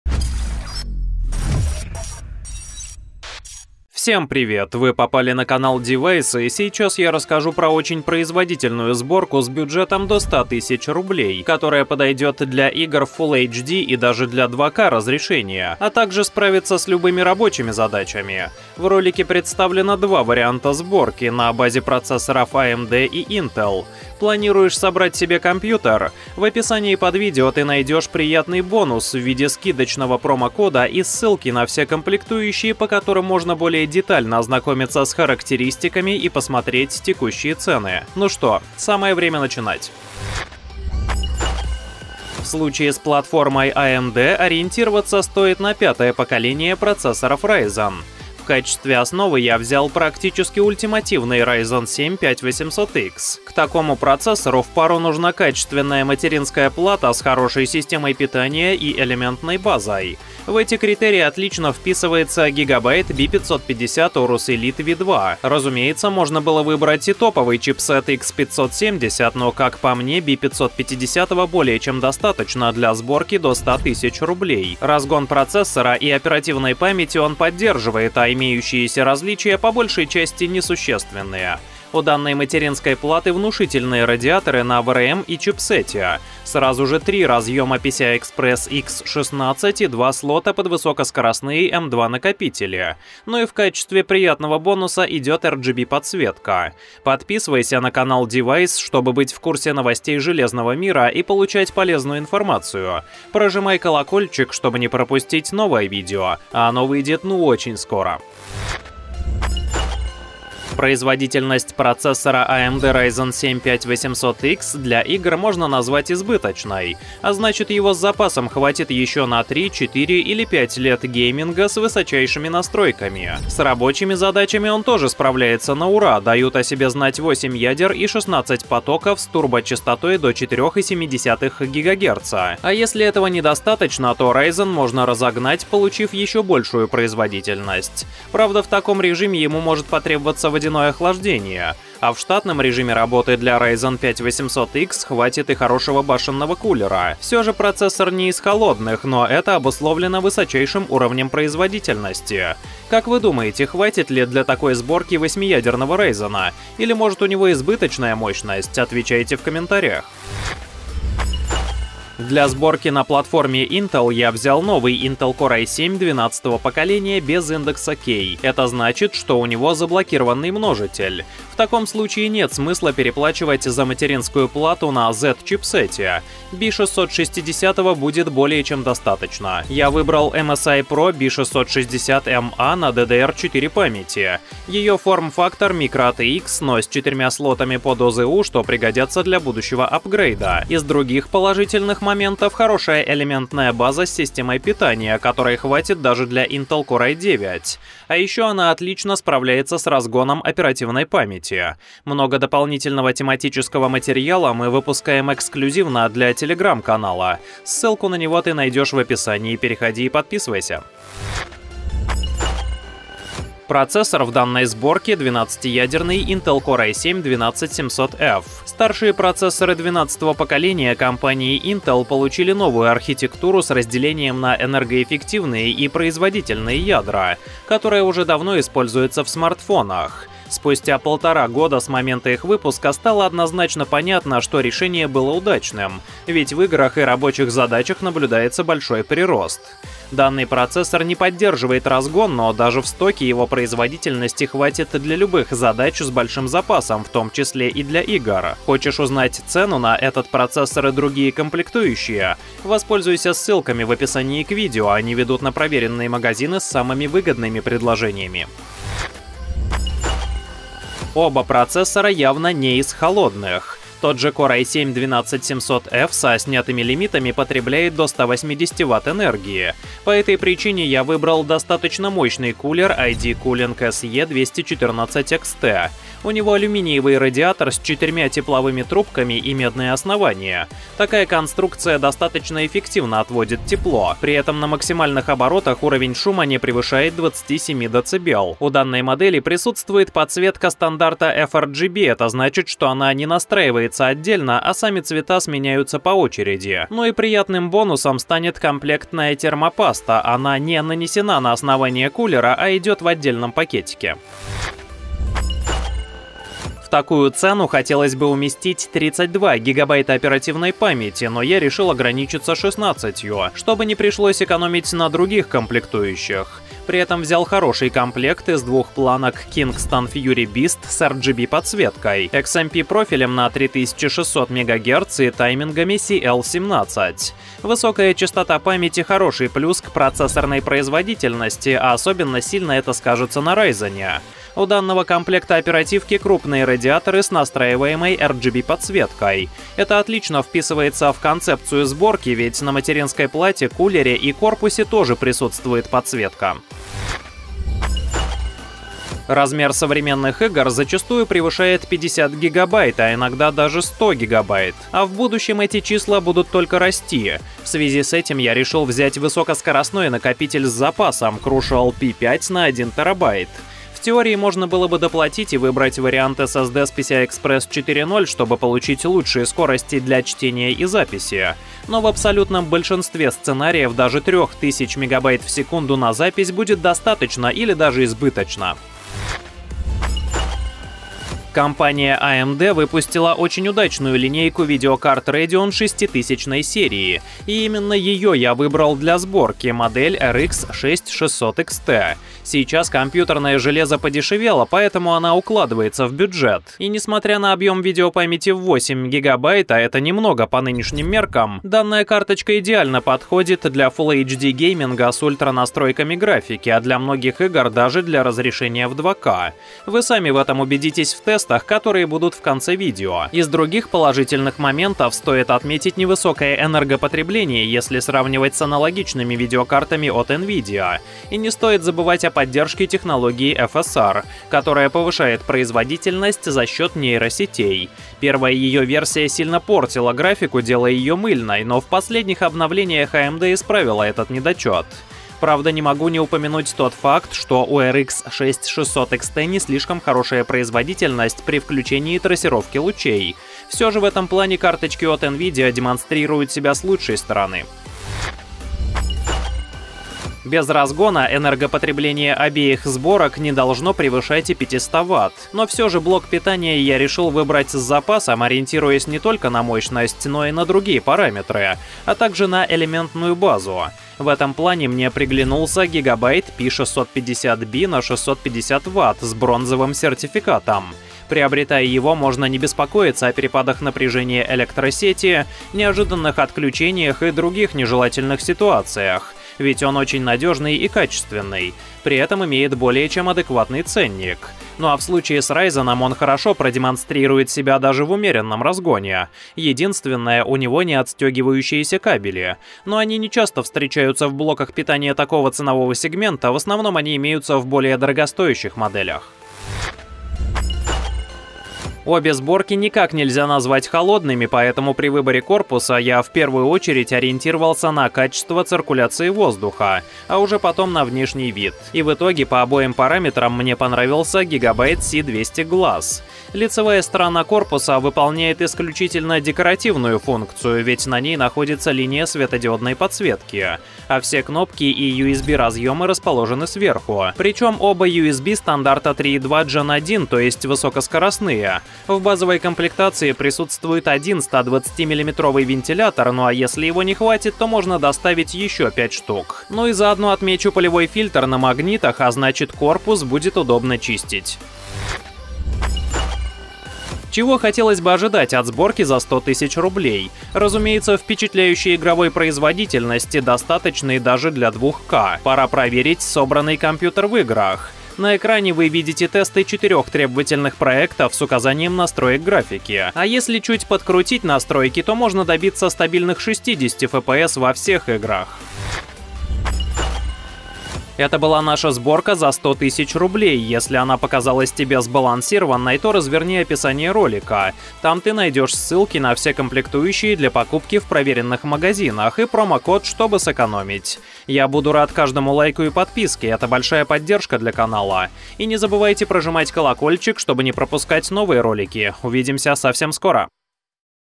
Сборка ПК за 100000 рублей. Октябрь 2022 года. Мощный игровой компьютер на Intel & AMD
✔ 00:00 Материнская плата (для AMD) GIGABYTE B550 AORUS Elite V2

02:10 Процессор AMD Ryzen 7 5800X

02:59 Материнская плата MSI PRO B660M-A DDR4

03:39 Процессор Intel Core i7-12700F

04:37 Кулер ID-COOLING SE-214-XT

05:27 Оперативная память Kingston FURY Beast RGB 16 ГБ (KF436C17BBAK2/16)

06:02 SSD Crucial P5 1 ТБ M.2 CT1000P5SSD8

06:38 Видеокарта AMD Radeon RX 6600 XT

07:29 Блок питания GIGABYTE P650B 650W

08:20 Корпус GIGABYTE C200 GLASS